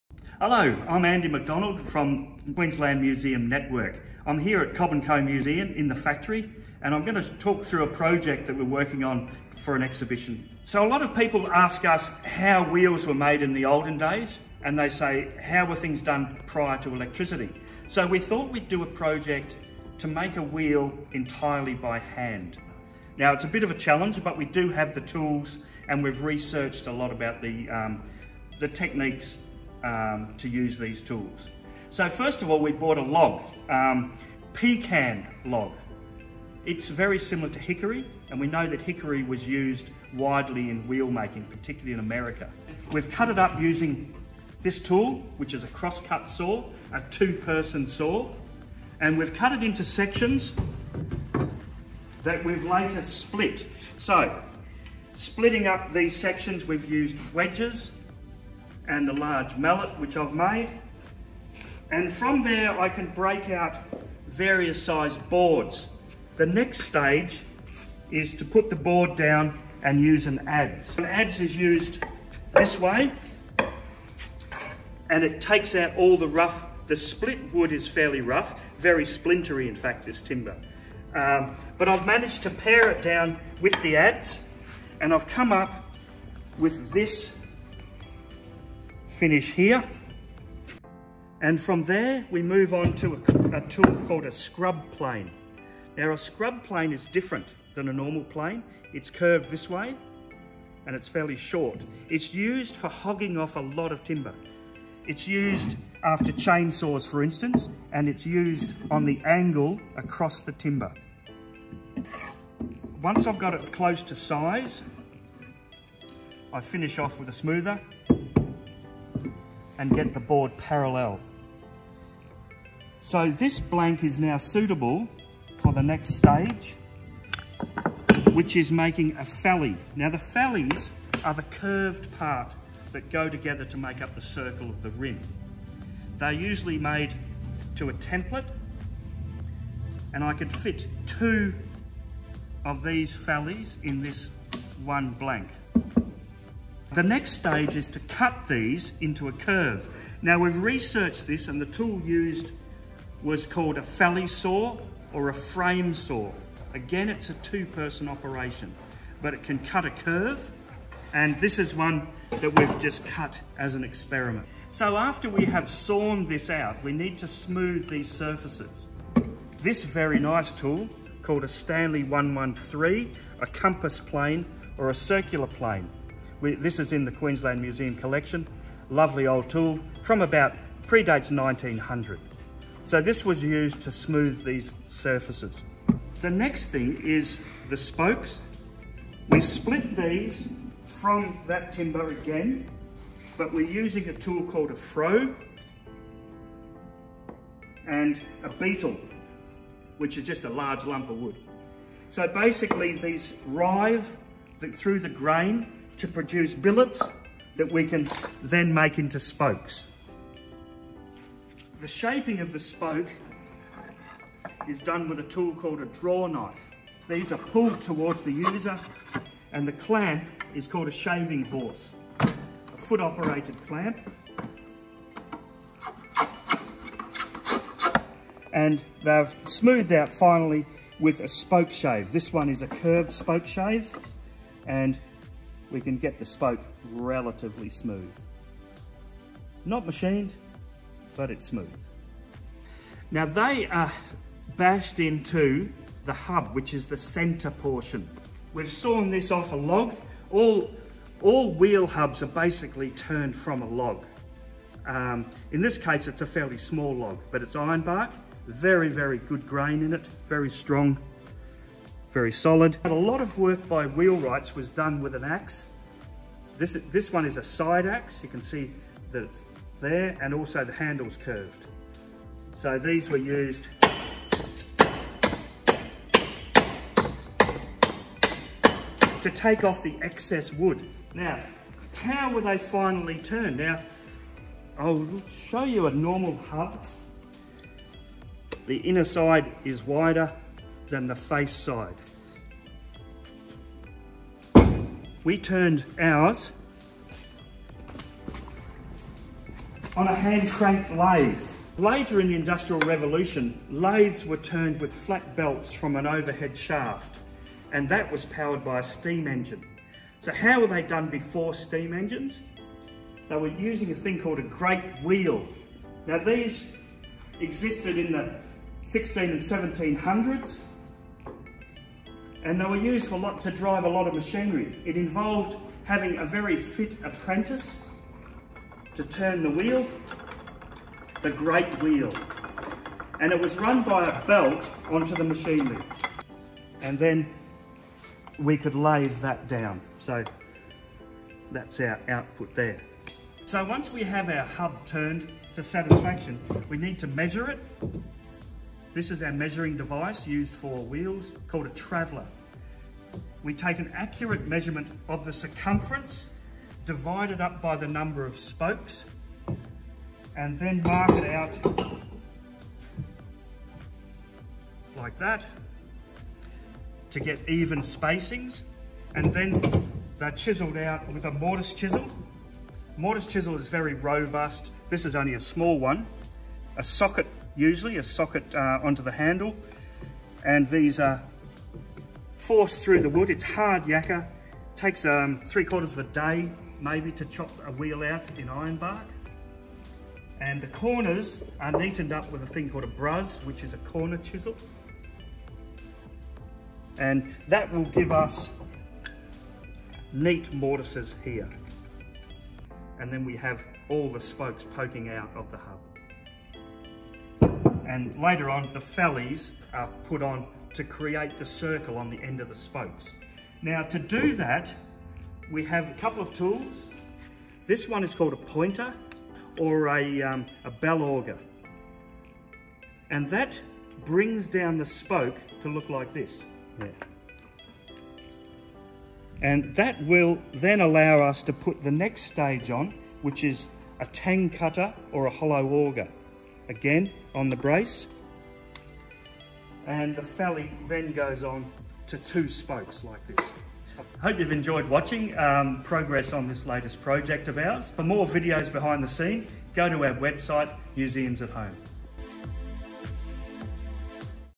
Andrew MacDonald takes us through the process of making a wheel entirely by hand | Cobb+Co Museum
Today our CouchCurator Andrew MacDonald, Factory Supervisor at @cobbandcomuseum takes us through the process of making a wheel entirely by hand, showing all of the tools that are used along the way.